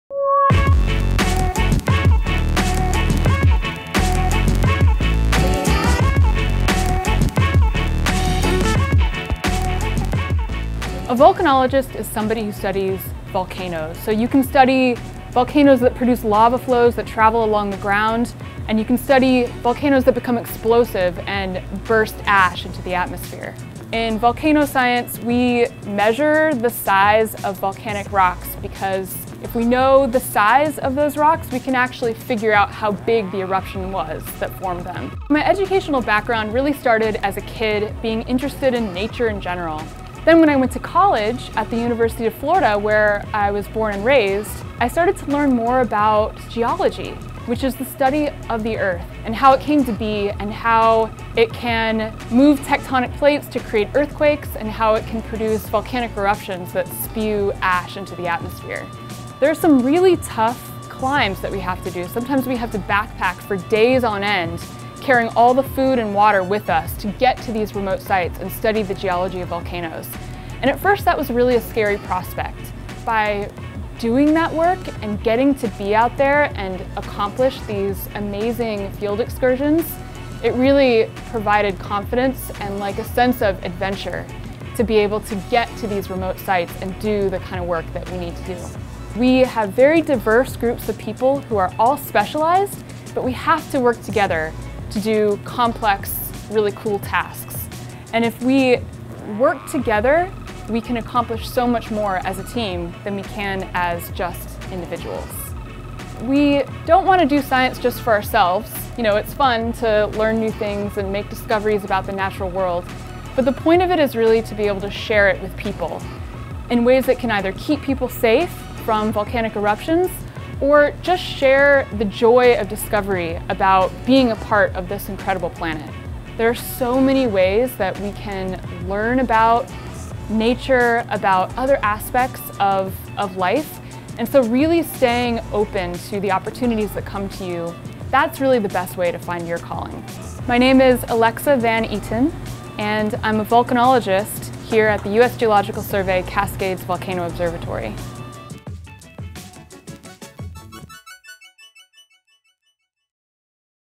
"We don't do science just for ourselves" Alexa Van Eaton Volcanologist: My Path
Do you know what a volcanologist is? It may sound like some sort of made up sci-fi job but volcanologists are in the business of keeping people and property safe. In this My Path Alexa Van Eaton talks about her work with volcanoes in the Pacific Northwest and how scientists keep on top of the all the shaking and quaking.